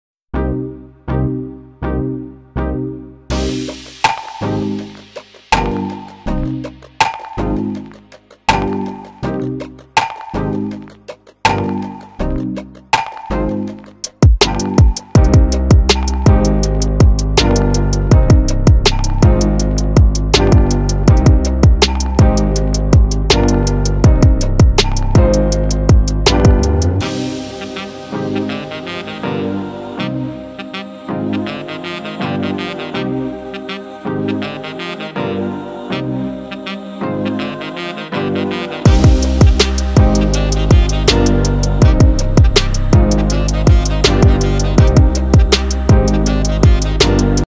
Over Excited Puppy
This puppy is so energetic that he is over-excited, walking him exhausted me.
While I was shivering from the icy cold weather, he was enjoying his late-night walk and eating snow.